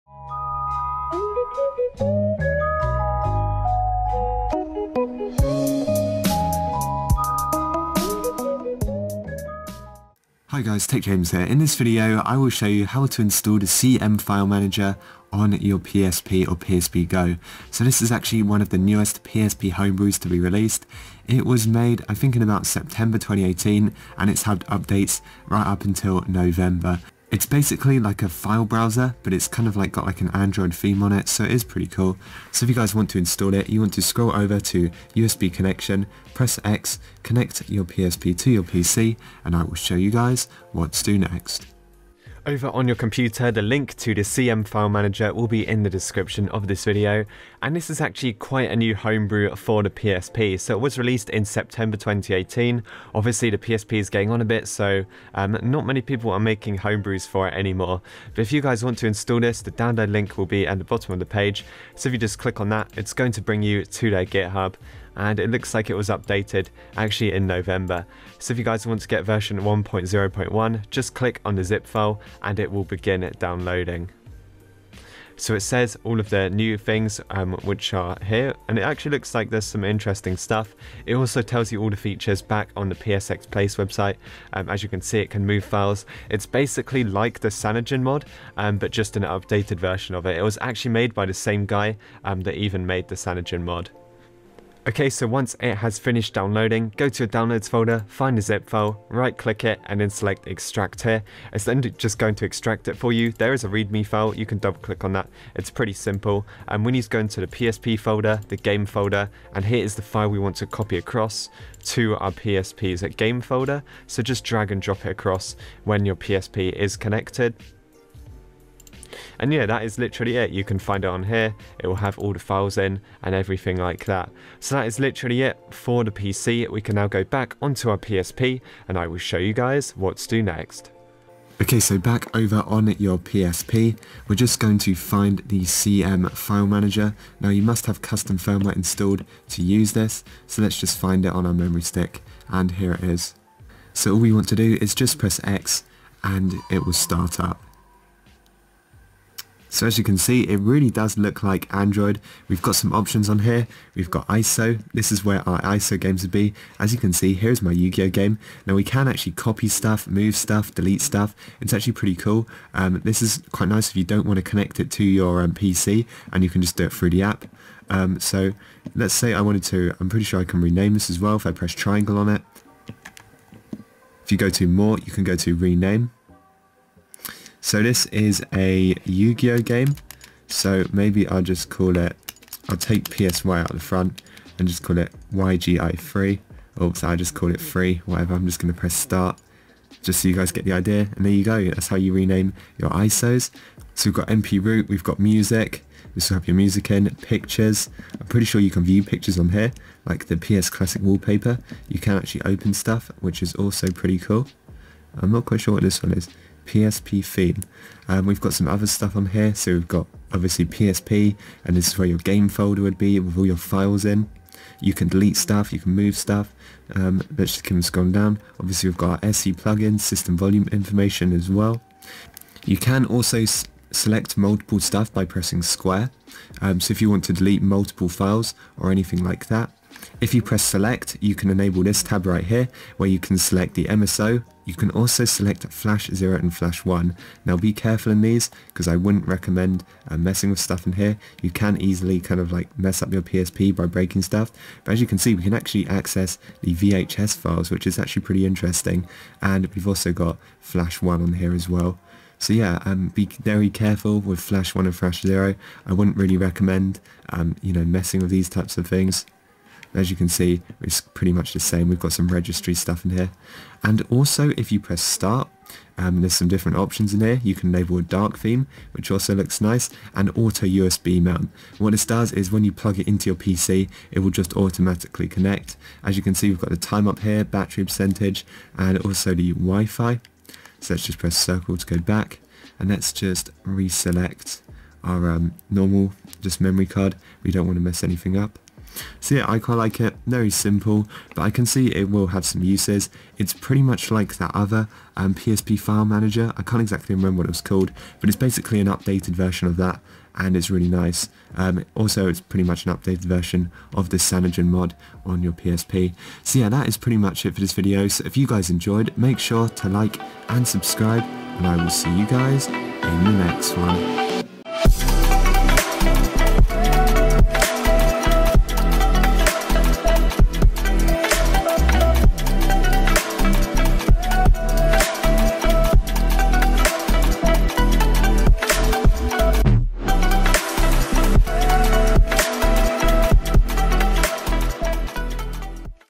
PSP How To Install CMFileManager! (Best File Manager App)
In this video, I’ll show you guys how to install the CMFileManager Homebrew application on your PSP or PSP GO. This is the latest and best file manager you can get for PSP’s.